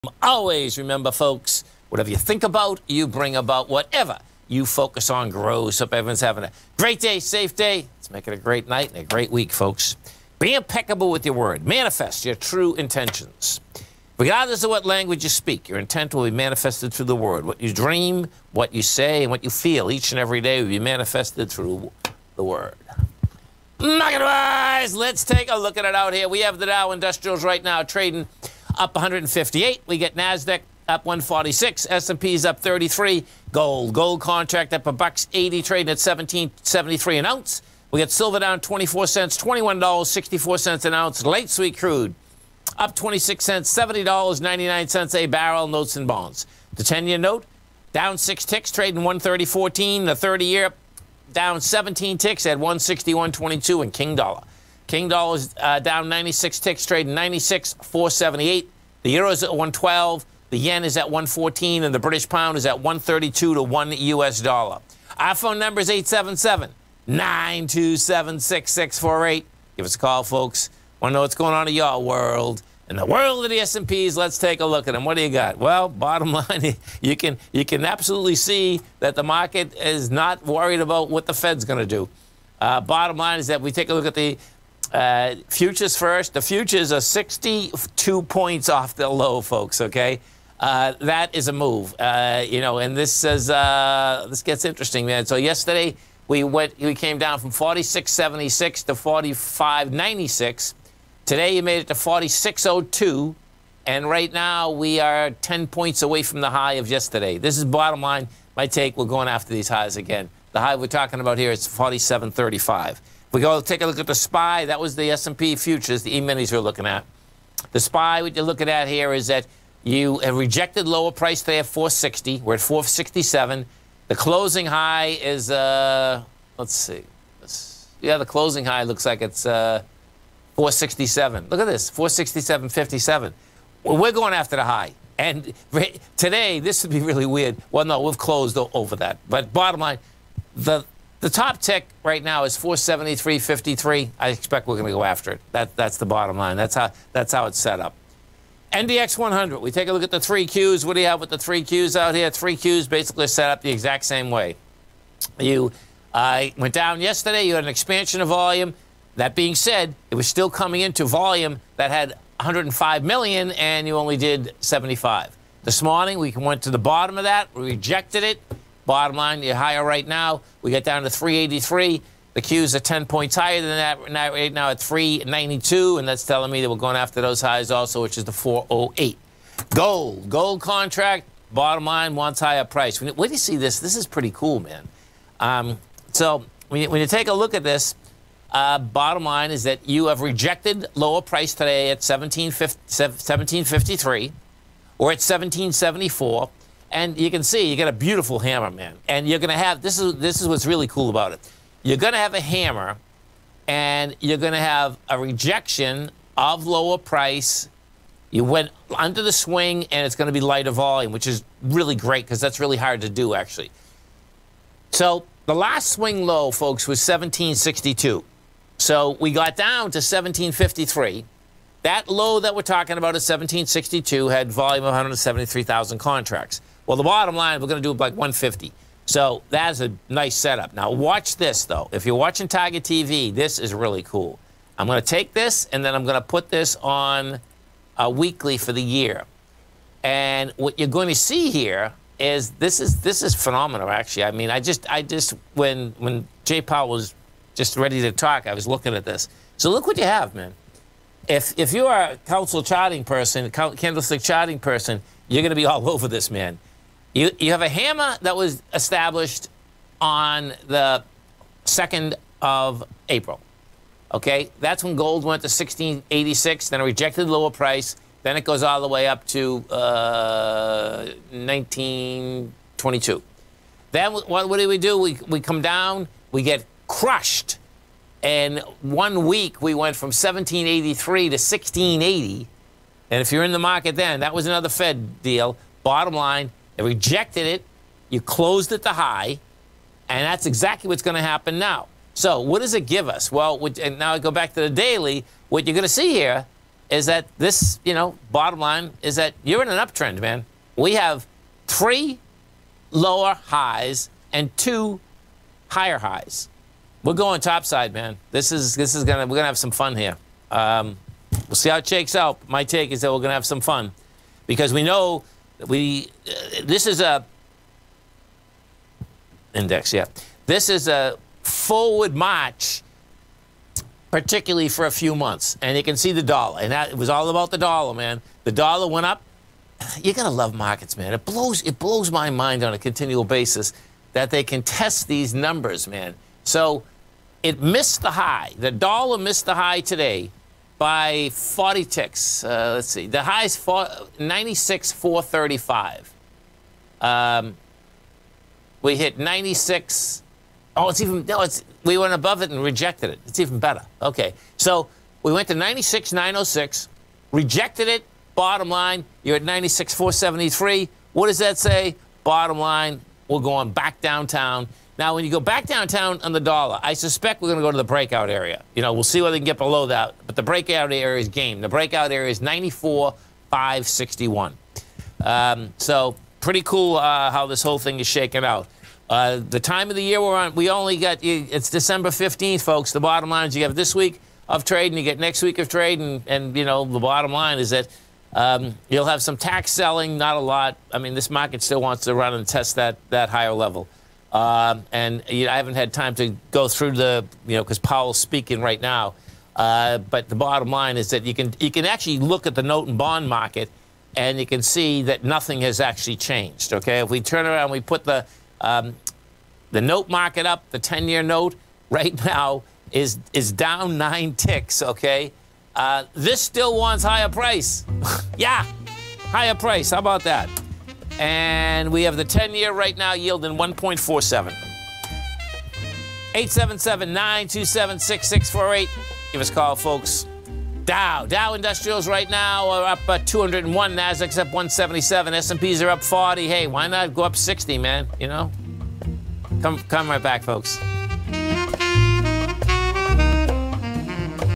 December 15th, Daily Market Recap - 2021
The Daily Market Recap brings you the latest and most accurate information on market conditions for that day.

9:06 The Morning Market Kickoff with Tommy O’Brien
10:06 The Tiger Technician’s Hour with Basil Chapman
11:00 Trade What You See with Larry Pesavento
12:06 TD Ameritrade’s Thinkorswim with Kevin Hincks and Tom White
1:06 The Trader’s Edge with Steve Rhodes
2:06 The Power Trading Hour with David White
3:06 The Tom O’Brien Show